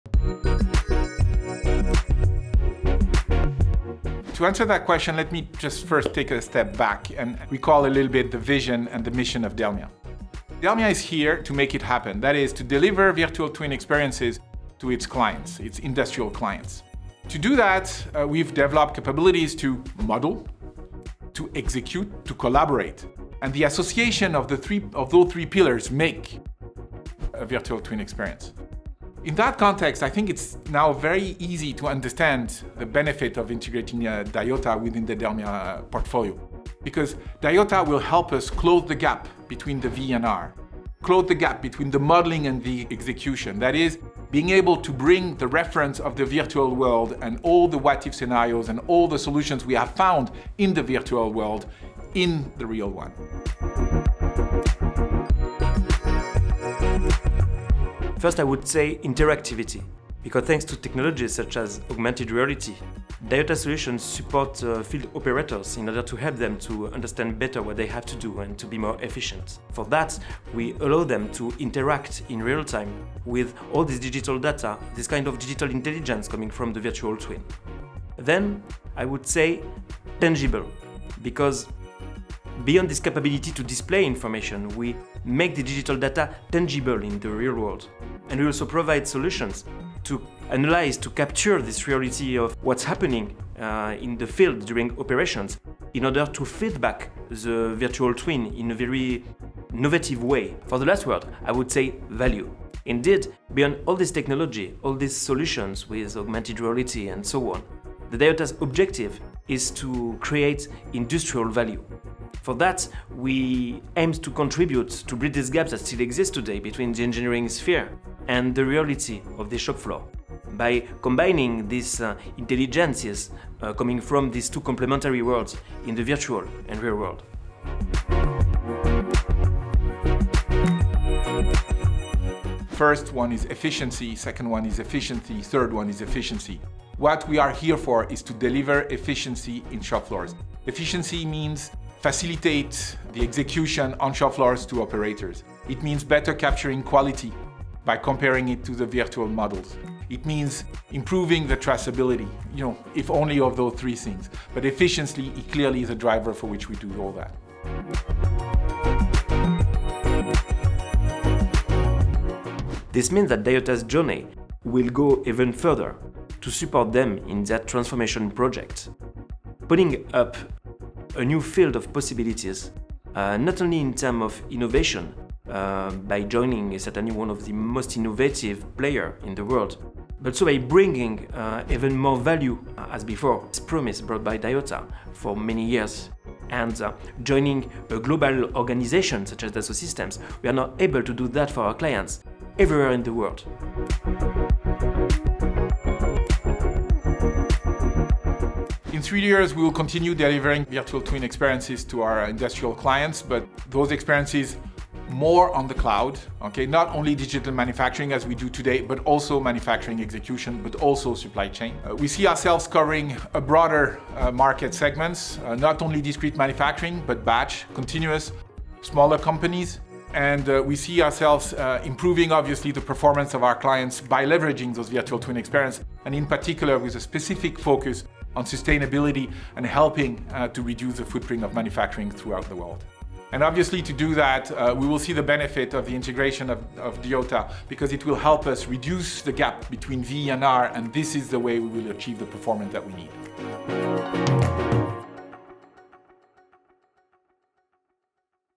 DELMIA x DIOTA - How's the future look like?
Guillaume Vendroux, DELMIA CEO, and Lionnel Joussemet, DIOTA CEO, answer some questions for us about the benefits of the addition of DIOTA to the DELMIA portfolio.